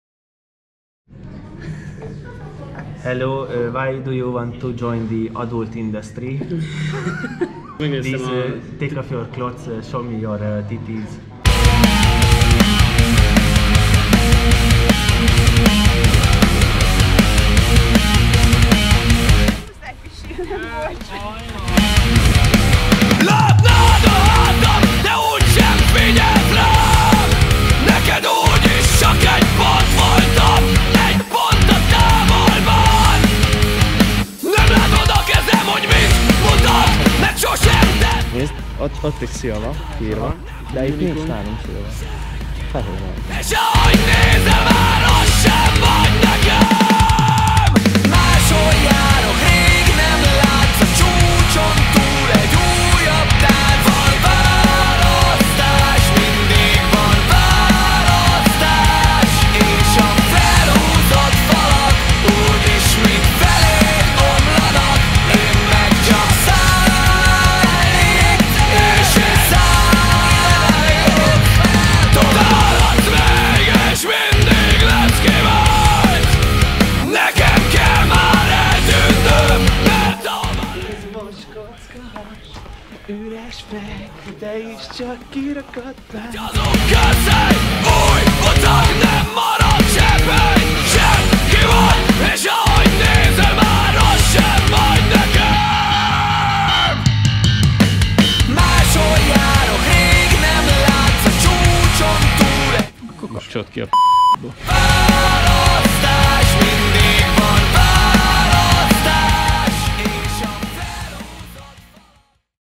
MIRROR 2021 őszi mozgolódás #2
Életképregény a Mirror 2021-es őszi országjárásáról.